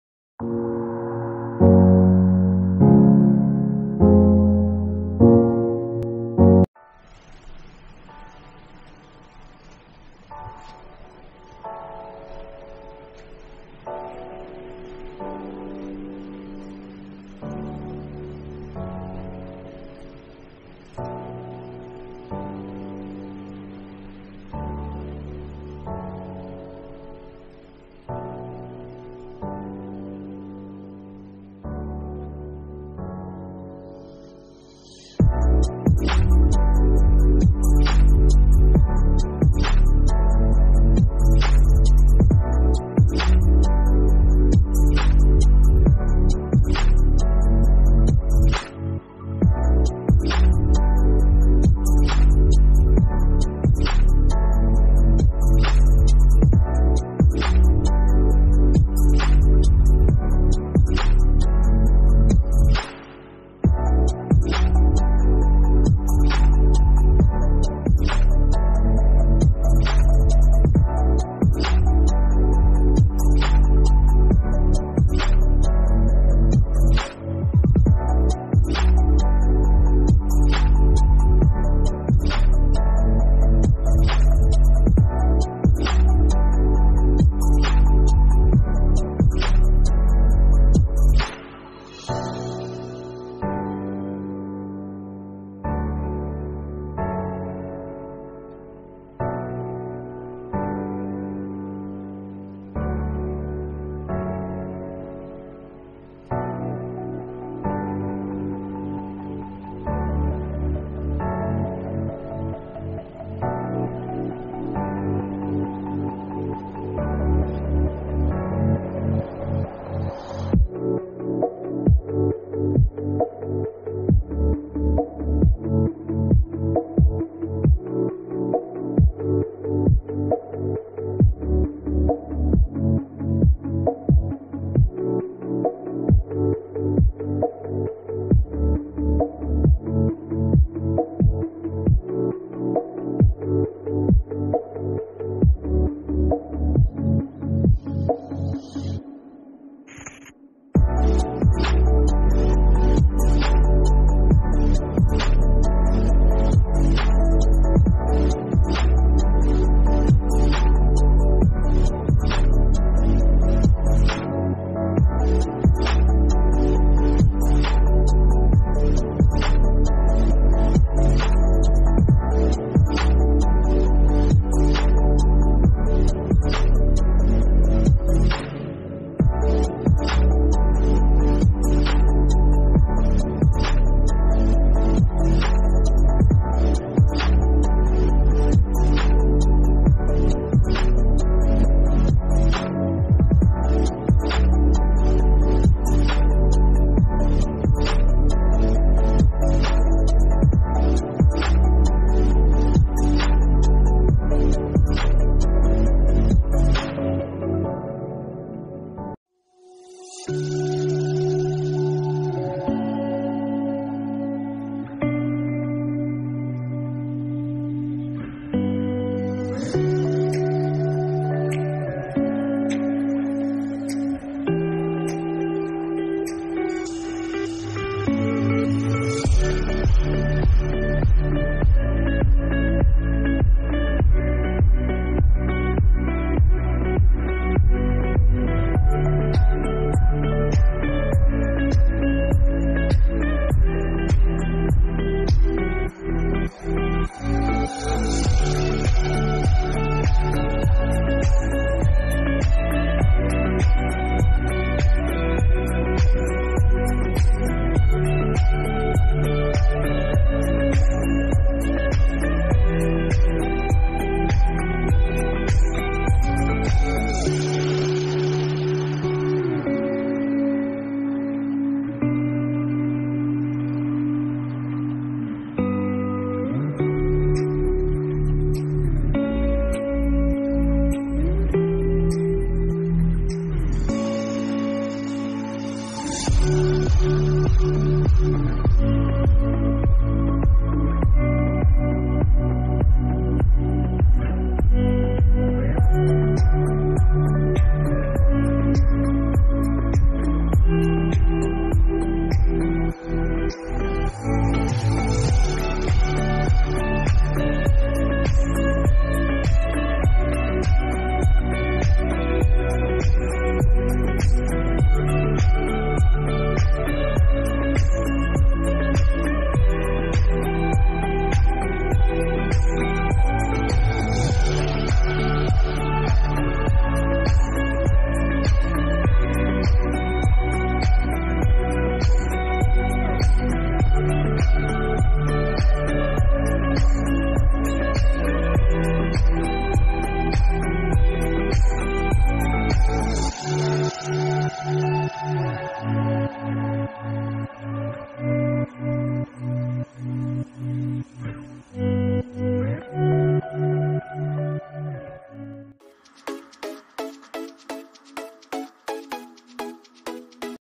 Village scenery with oil pastel colour||How to draw scenery with pastel colors.
Village scenery with oil pastel colour.
How to draw scenery with pastel colors.